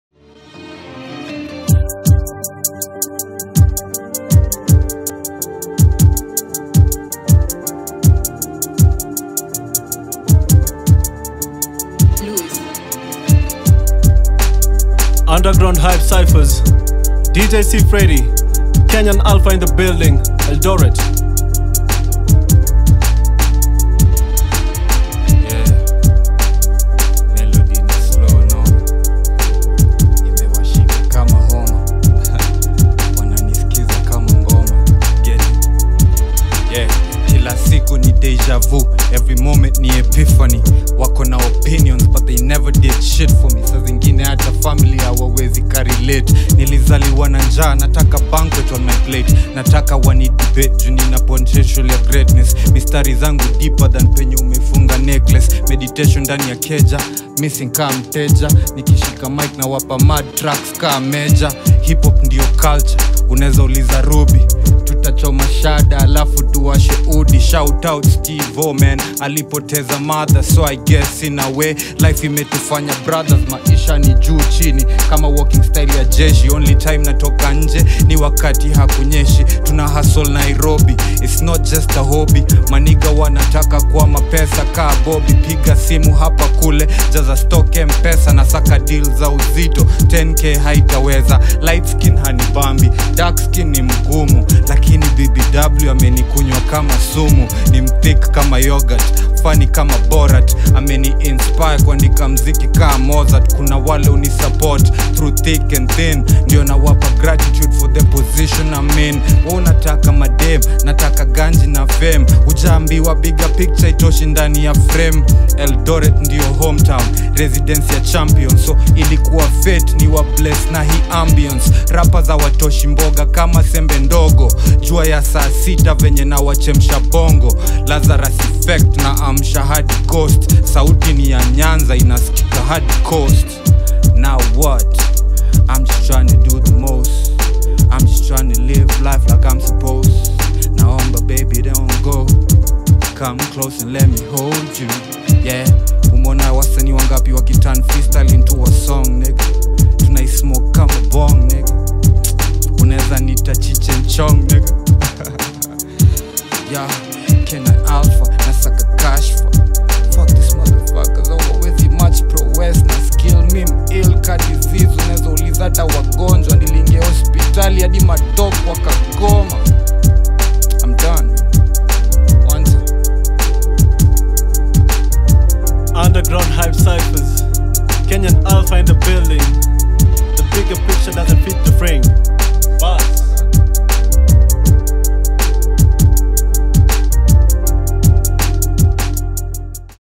KENYAN ALPHA | PART 2 | UNDERGROUNDHYPEKe | FREESTYLE RAP 2018
Kenyan Alpha is an Underground artist in Kenya, from Eldoret. Watch his freestyle!! PART 2